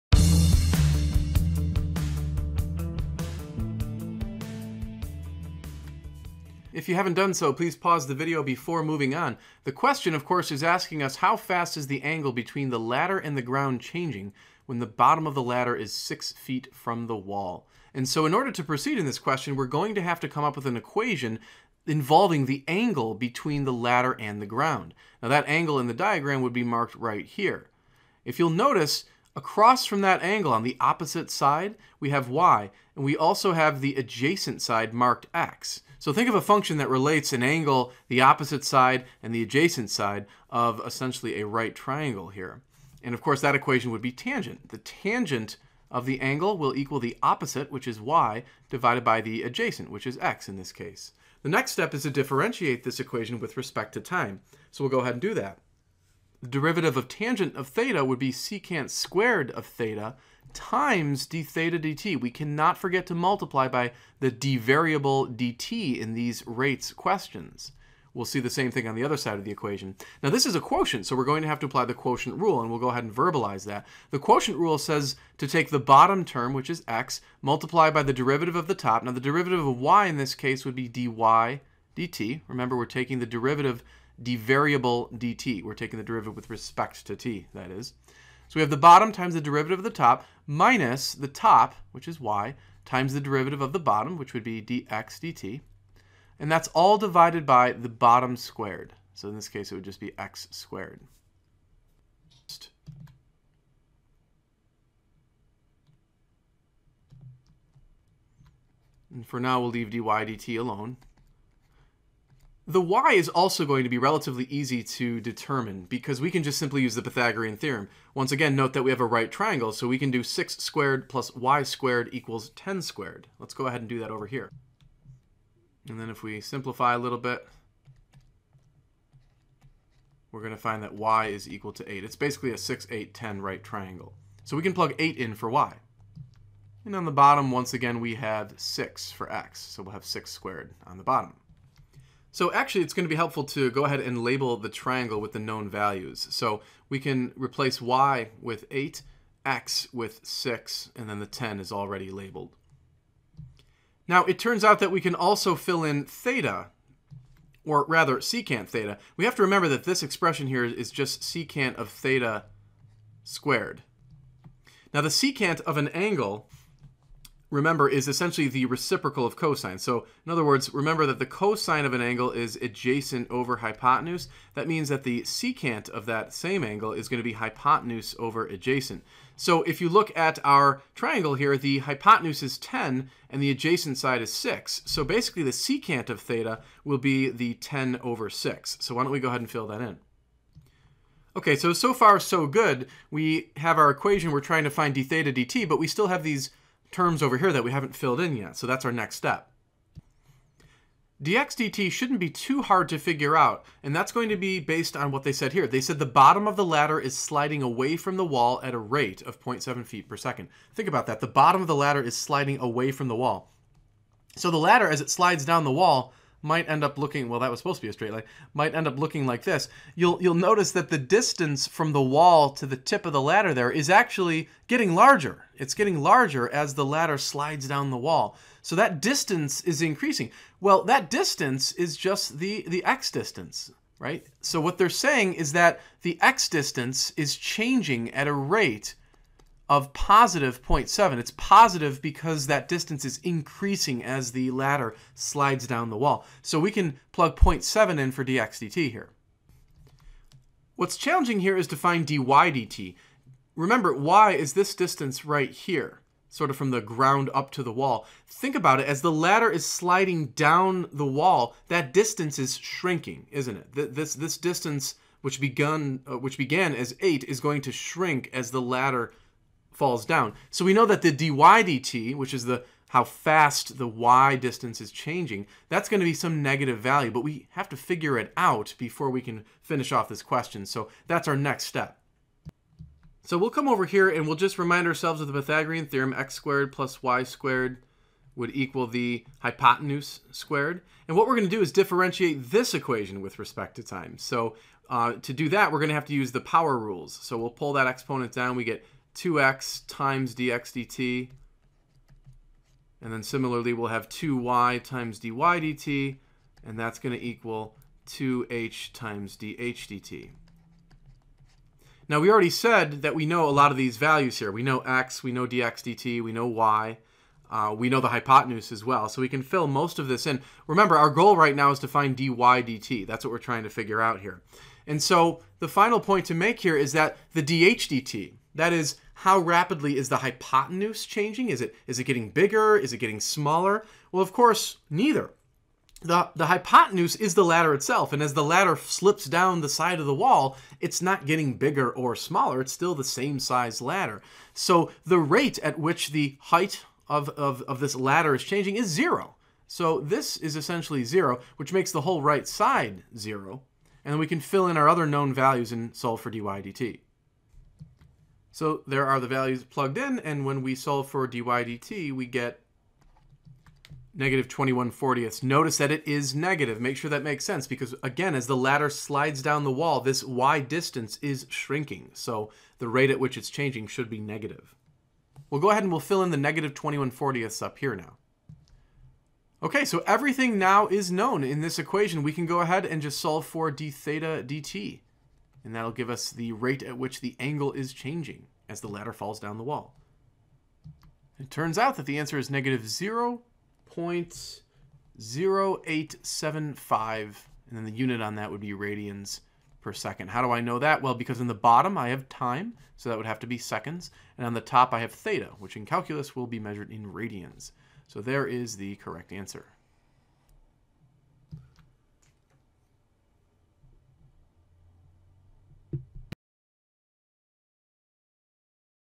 A ladder 10 ft long rests against a vertical wall. If the bottom of the ladder slides away from the
A ladder 10 ft long rests against a vertical wall. If the bottom of the ladder slides away from the wall at a rate of 0.7 ft/s, how fast is the angle between the ladder and the ground changing when the bottom of the ladder is 6 ft from the wall? (That is, find the angle's rate of change when the bottom of the ladder is 6 ft from the wall.)